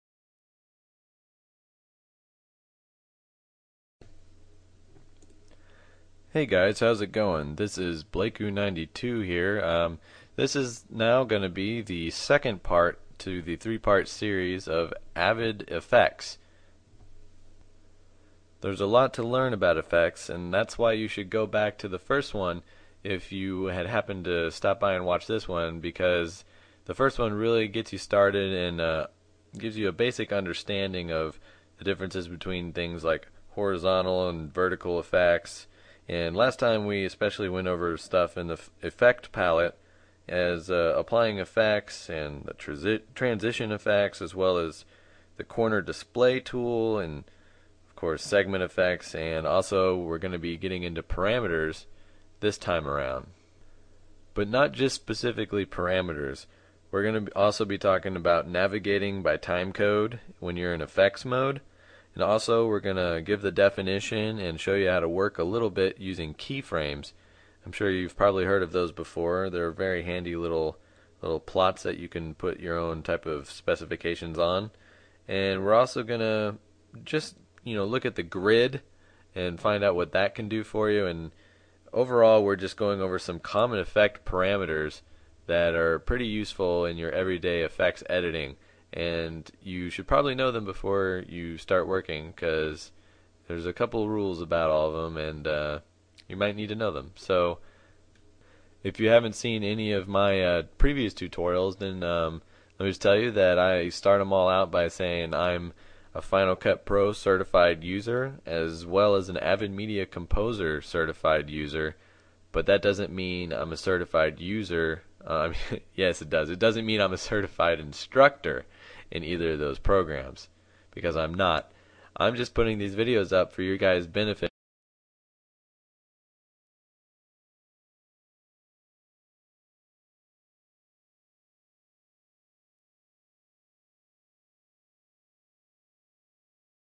Avid Effects Part 2
Part 2 of 3, this class teaches the basic concepts of how Avid Media Composer's Effect Mode operates.